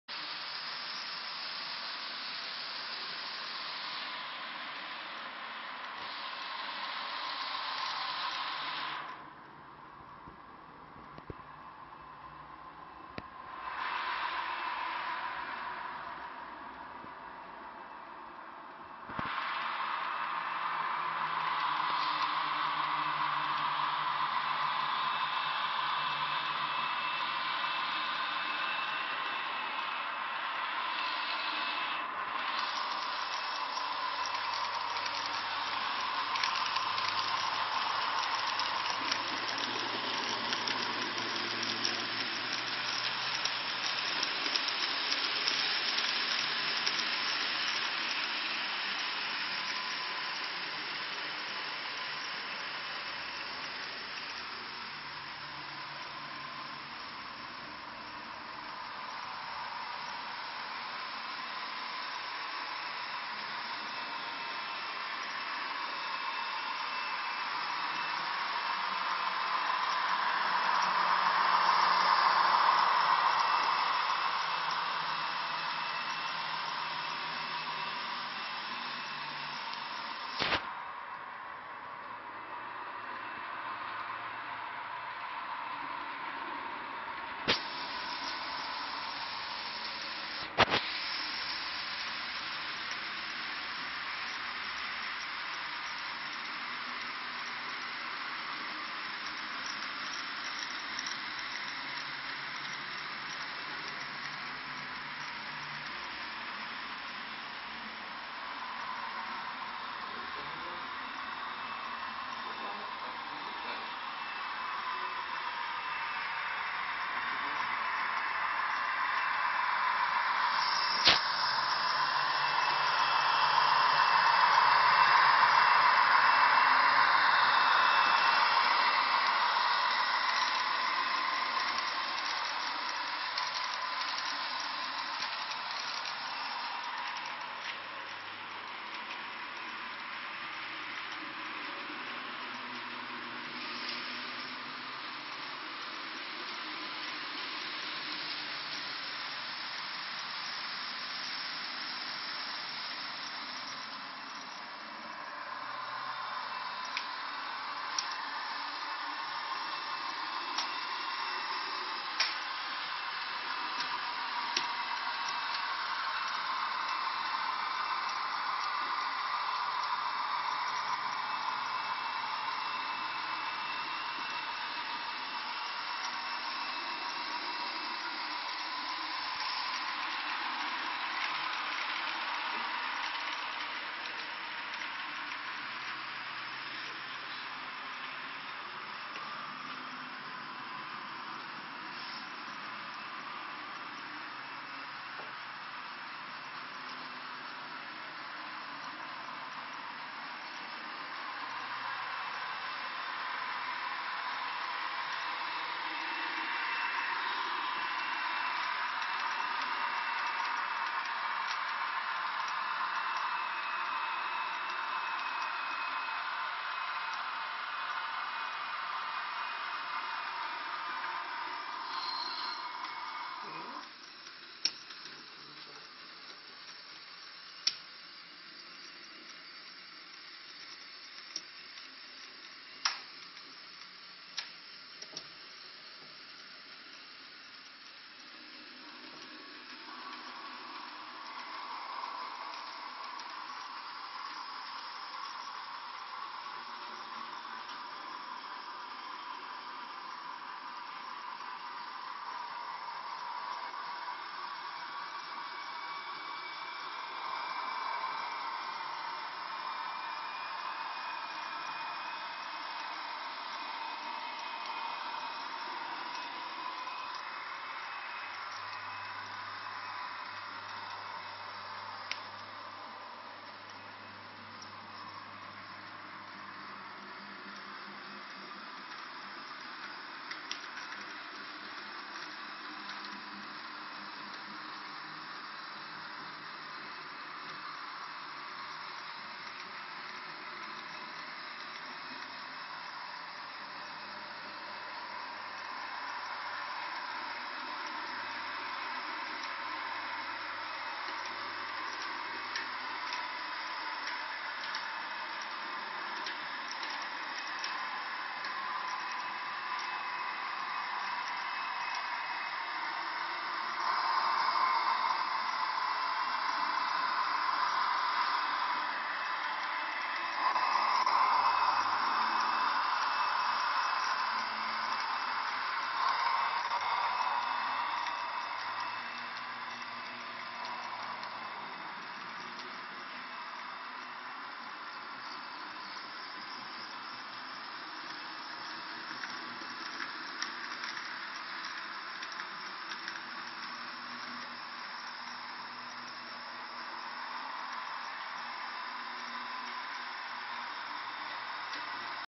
TT kolejiště. Digitální a analogový provoz.
Modelové kolejiště TT. Kombinace analogového a digitálního řízení kolejiště. Na videu analogový provoz.